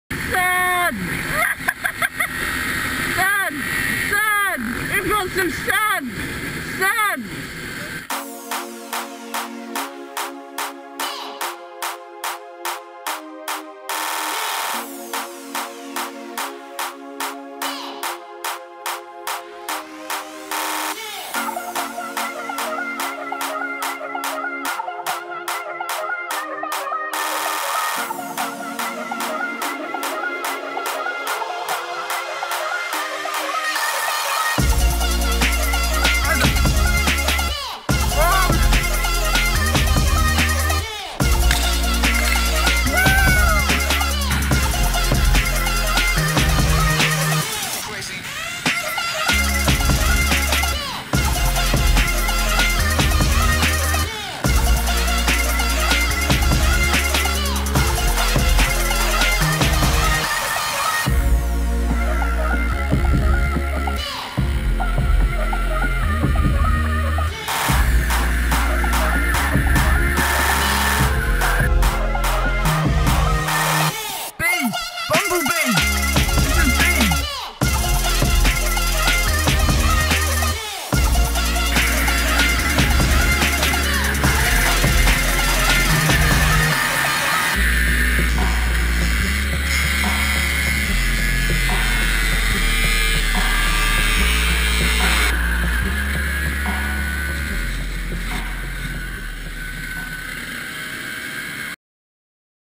SUNDAY MEAT #1 - Supermotos and Transformers
Today we saw a transformer and a limited edition Vauxhall Corsa. sub